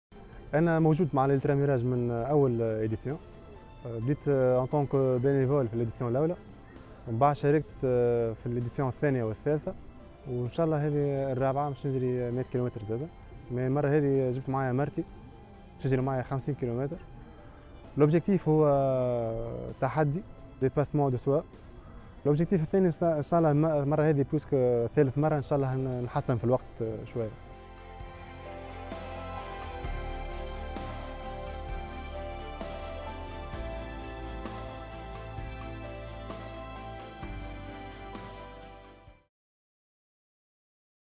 Ultra Mirage El Djerid 100km & 50km - Interview Foued Ben Hmida
Ultra Mirage El Djerid  • 2020 October 3rd • 2 singles stages ultra trails 50 km & 100 km across Sahara Desert • Tozeur • ITRA Member + UTMB® qualifier